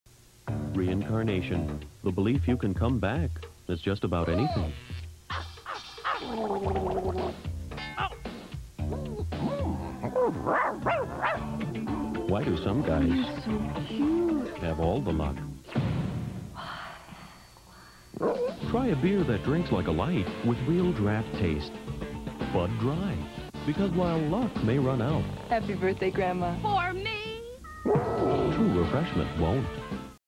1992 - Bud Dry - Why Ask Why? (Reincarnation) Commercial
The premise of reincarnation is explored using a variety of different objects. A lamp, toy monkey, statue, carnival-shooting-duck-game-thing and puppy are displayed as potential destinations for the afterlife. Since an attractive woman ends up adopting the puppy, that's presented as the best case scenario. In a twist, it turns out that the woman had never intended to keep the dog herself. Rather, she picked it up as a birthday gift for her grandmother, rendering that lifetime a far less appealing fate.